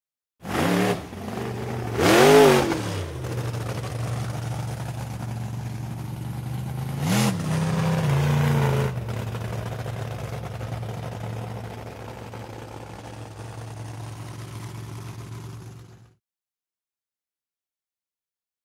Monster Truck Revving and Racing by Thru the Obstacle Course with Revs Take 9
Monster Truck Revving and Racing by Thru the Obstacle Course with Revs Take 9 · The Hollywood Edge Sound Effects Library

Dragsters & Monster Truck Sound Effects